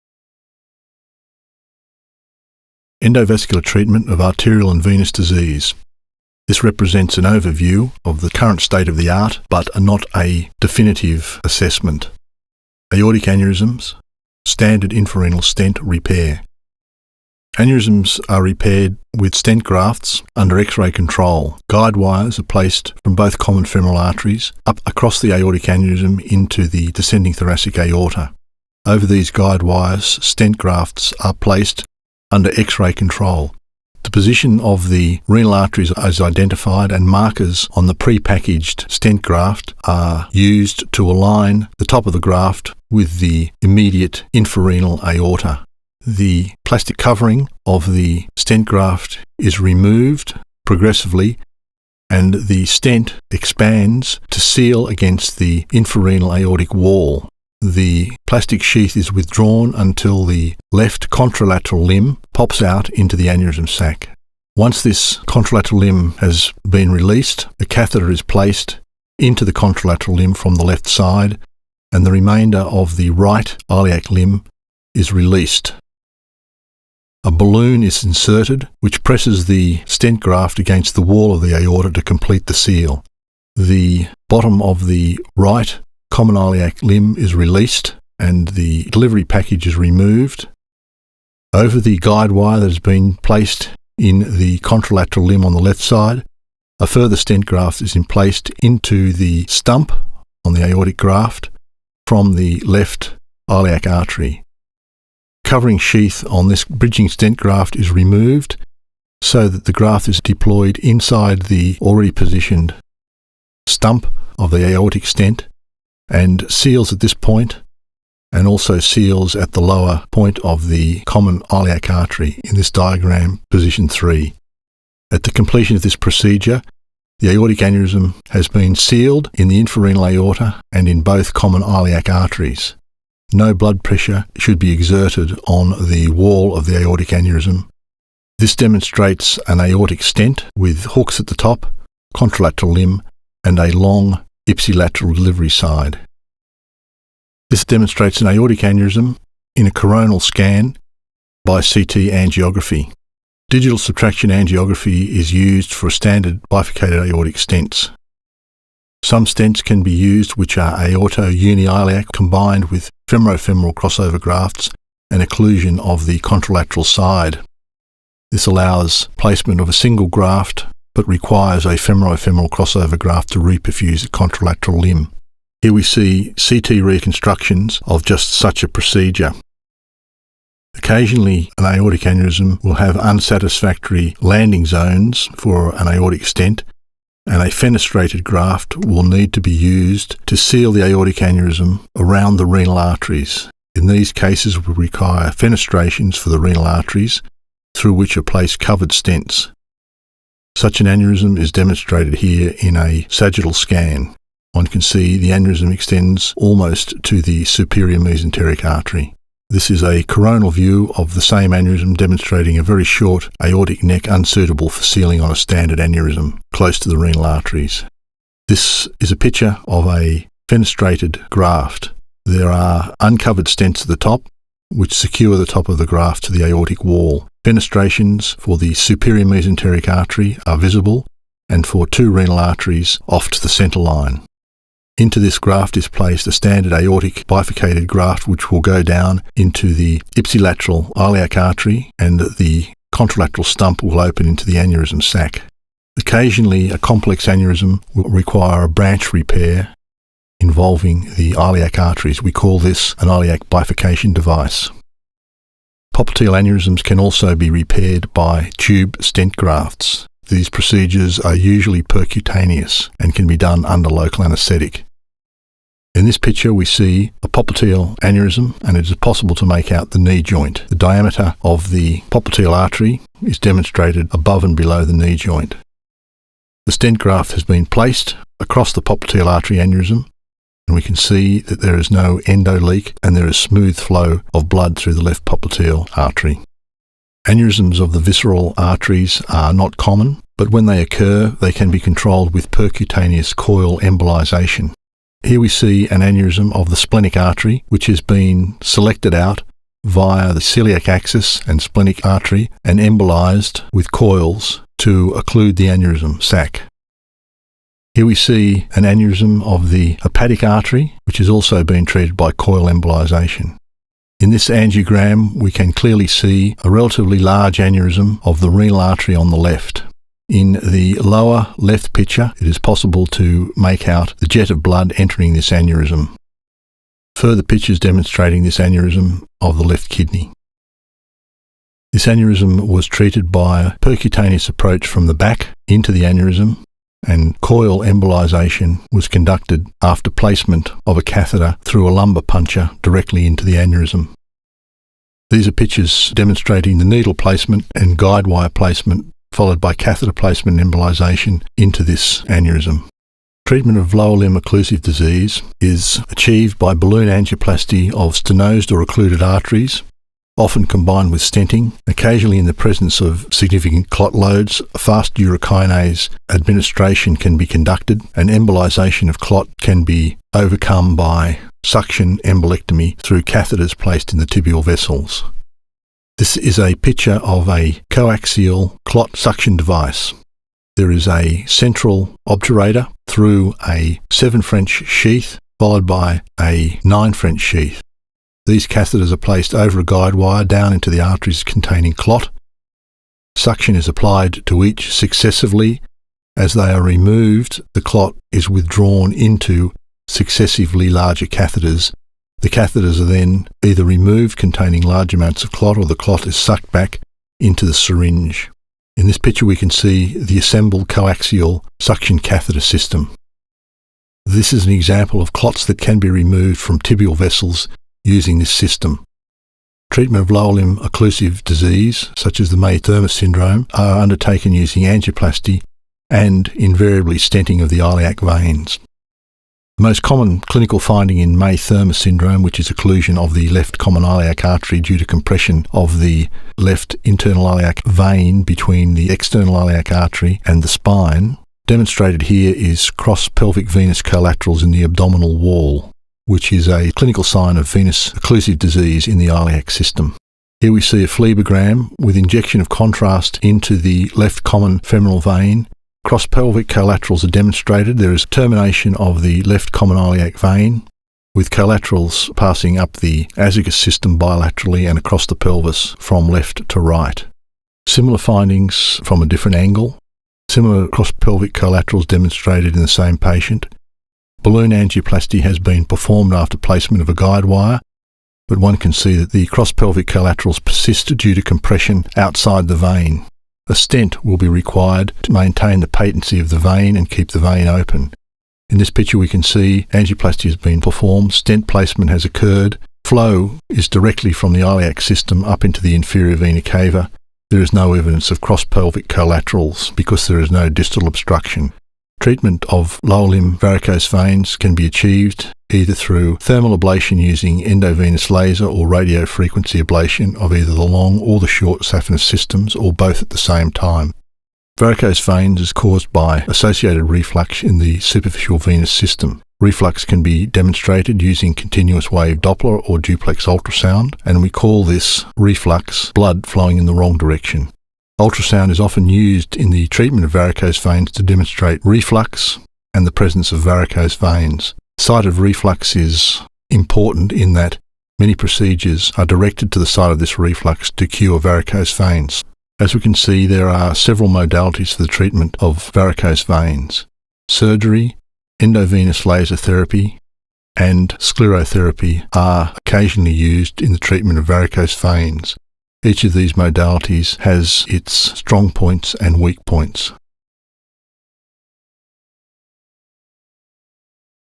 Endovascular Treatment for Arterial and Venous Disease
This video outlines the current state of the art, but not definitive, techniques for treating arterial and venous disease of the aorta and lower limbs. It is designed to give patients, medical staff and relatives some insight into how these techniques are employed. This video was produced for and narrated by Dr Raymond Englund. Dr Englund is a consulting vascular surgeon based in Sydney, Australia.